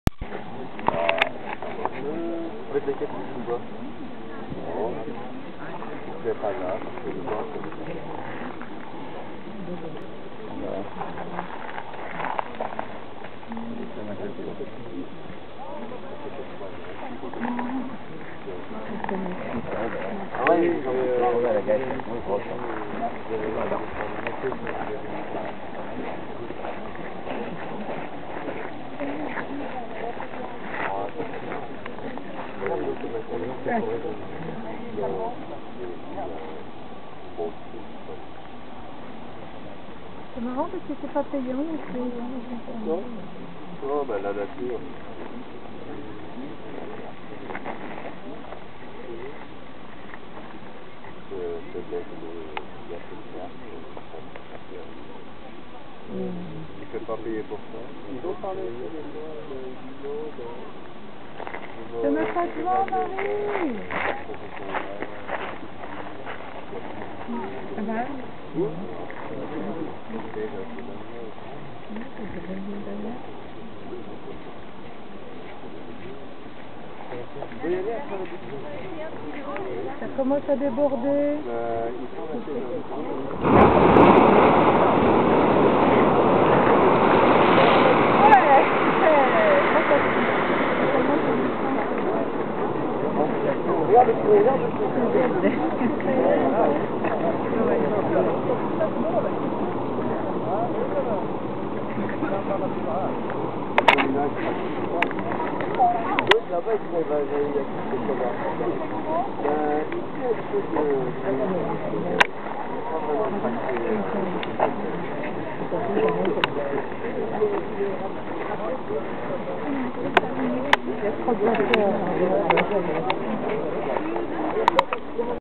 Islanda - Gran Geyser
sotto la pioggerellina islandese
attendiamo l'evento